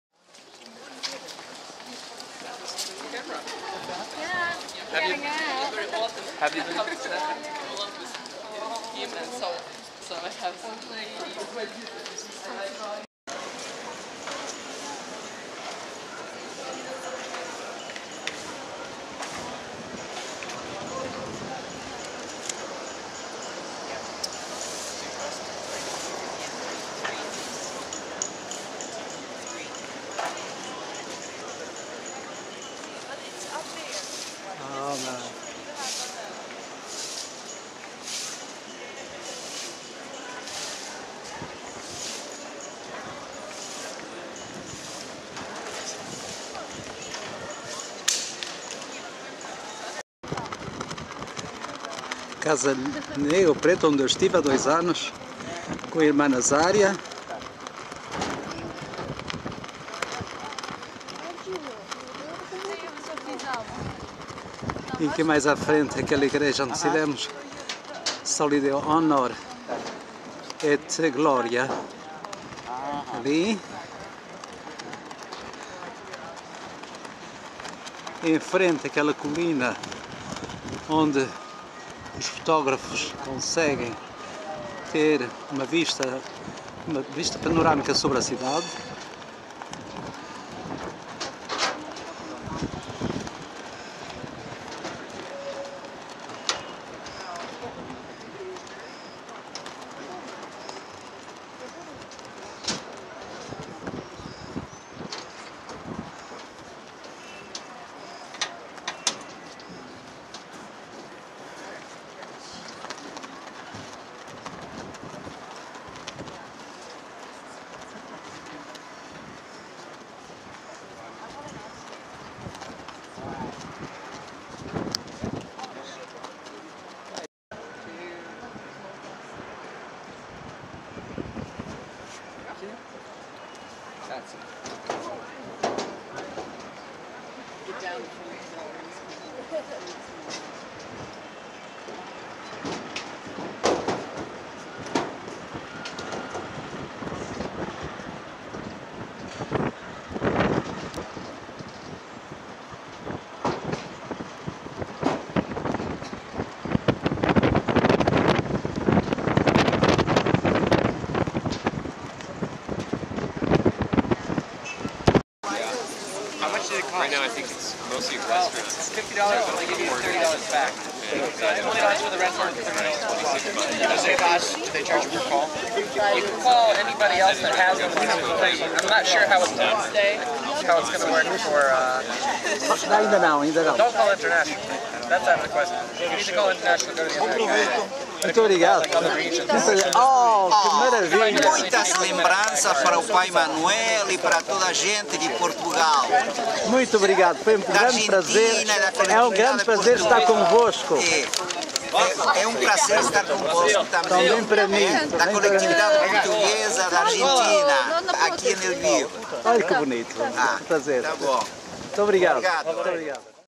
Погляд на Львів з Ратуші
Погляд на Львів з Ратуші студентами школи